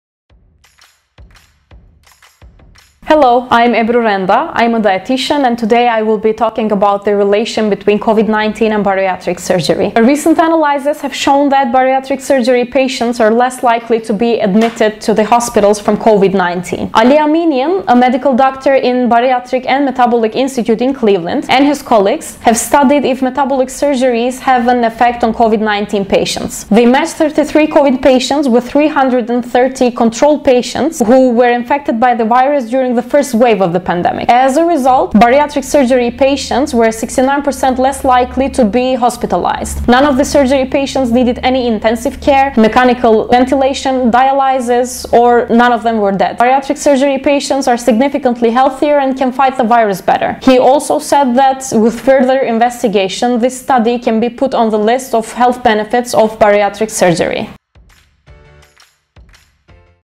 Zayıflama Ameliyatları ve Covid-19
Obezite ve metabolik cerrahi sonrası bağışıklık sistemi en çok merak edilen konulardan biri. Bariatrik cerrahinin COVID-19'a etkilerinin araştırıldığı bir çalışmayı sevgili diyetisyenimiz Ebru Renda sizler için anlattı👍

The state of immune system after bariatric surgery is one of the most frequently asked questions. Our dietitian Ebru Renda talked about a study about the effects of bariatric surgery on Covid-19👍